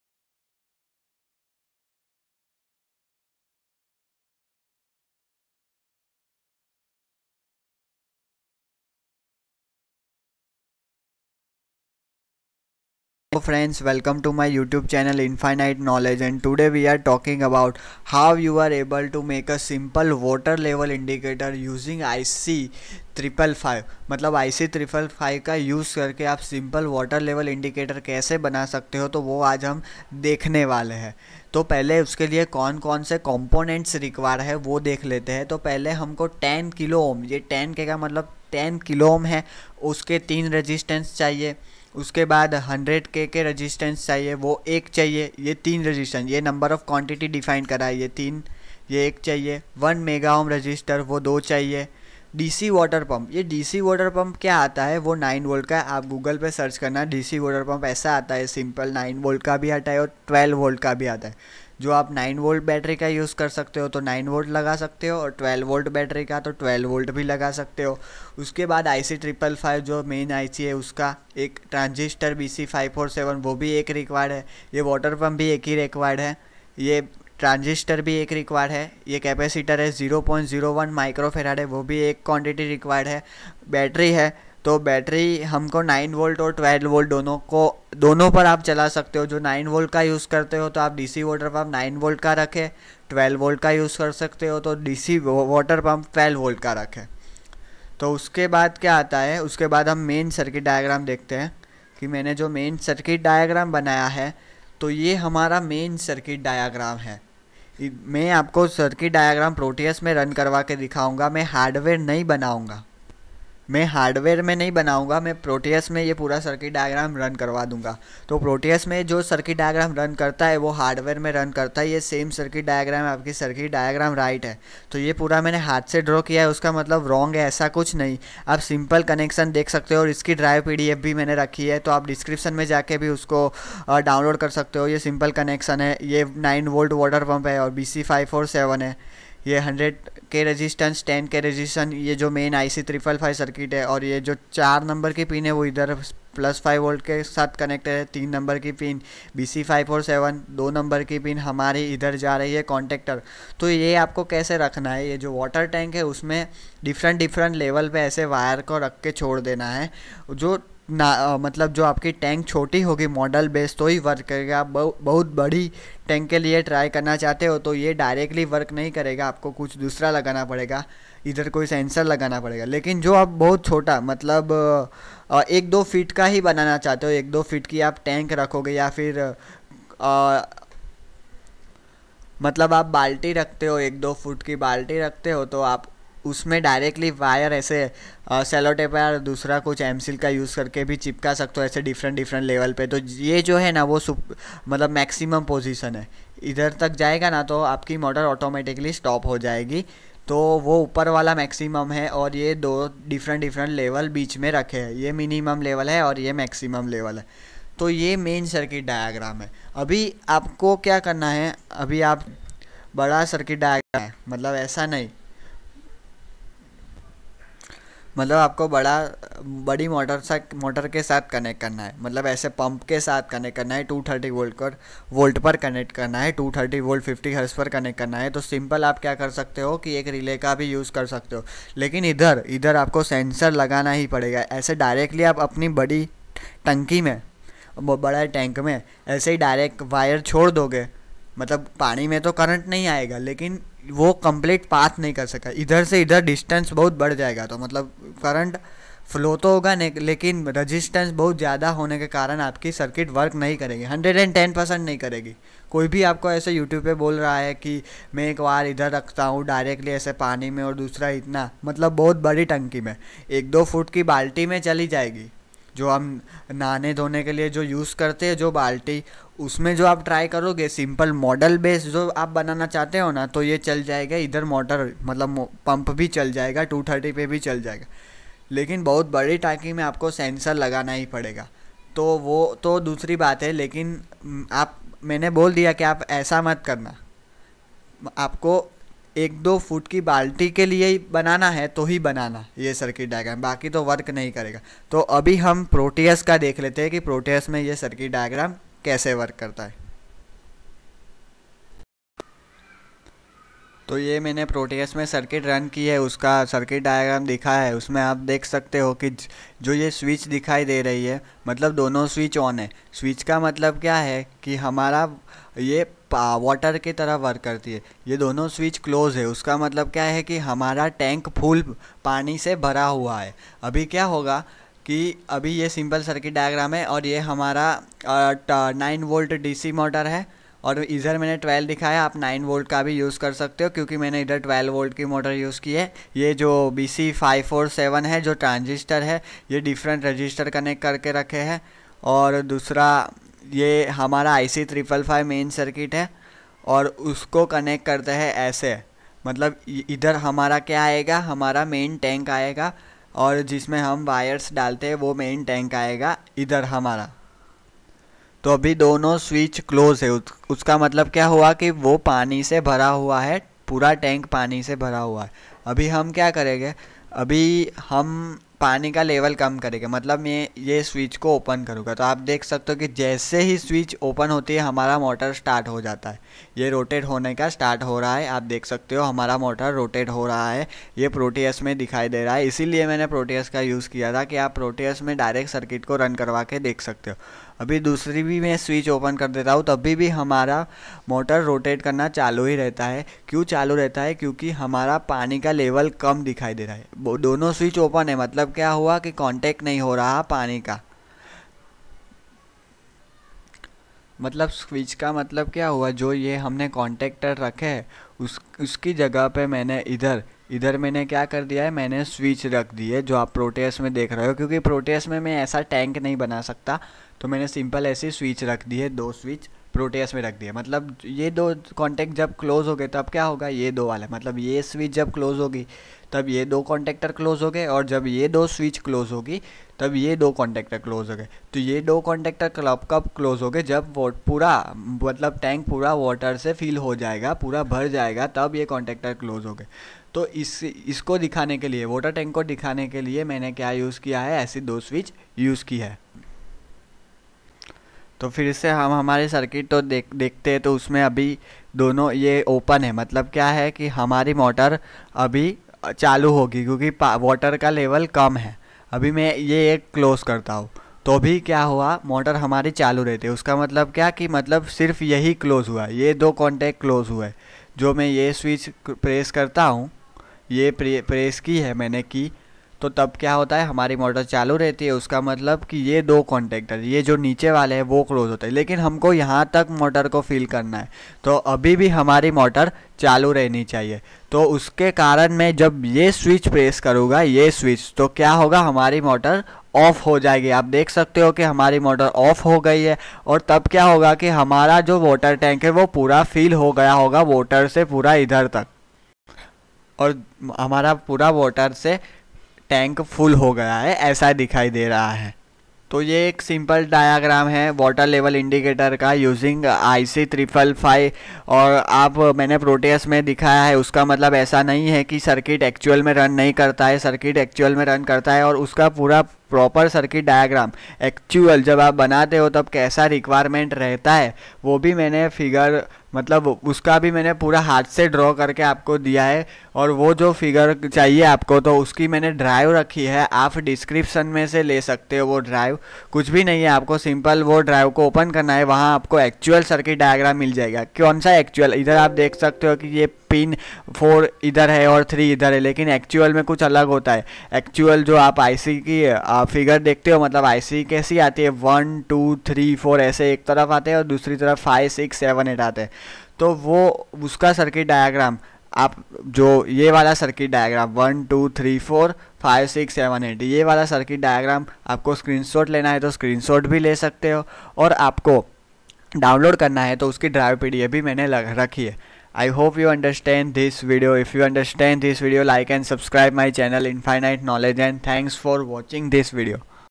Water level indicator using IC 555 || timer ||mini project || Proteus software
This video is helping you to understand how you are able to make the simple water level indicator by simple and less components.
I have shown circuit working in the proteus 8 software.